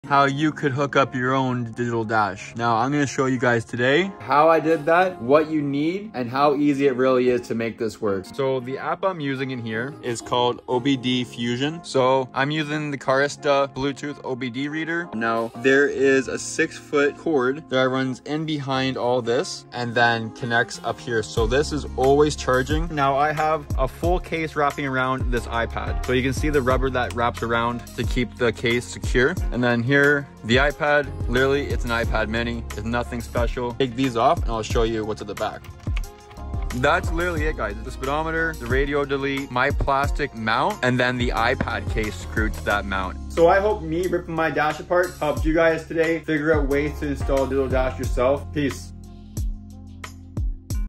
How To Setup A Digital Dash Is Now Live!
In this video i quickly give you a run down on how to get a digital dash setup in your car.